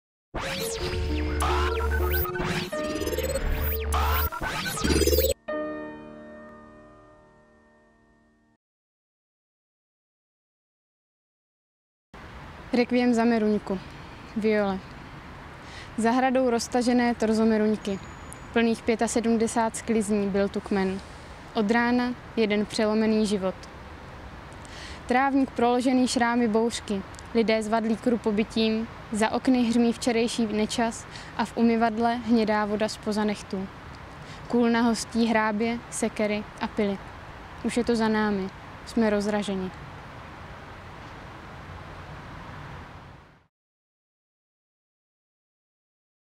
Deník Referendum - Nedělní chvilka poezie. Díl 51. - Alžběta Michalová: "Rekviem za meruňku"
Alžběta Michalová se narodila v roce 1991 v Praze. Studuje Gymnázium Matyáše Lercha v Brně. Básně publikovala na serveru Písmák, v internetové revue Atempo aj. Získala první místo ve studentské soutěži při Ceně Josefa Škvoreckého. Otištěné básně pocházejí z připravované sbírky s názvem Zřetelně nevyprávíš.